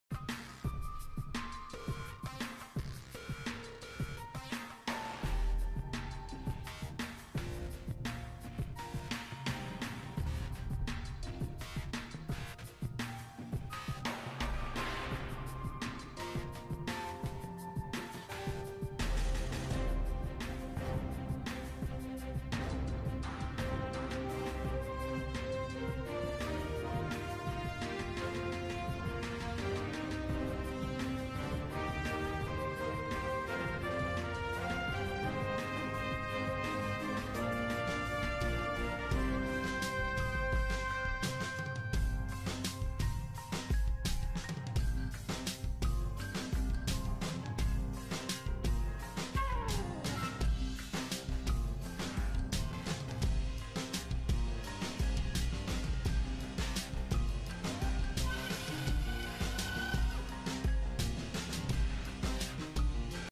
La Red se desconecta del sistema de medición de rating online de Kantar Ibope Media
La Red.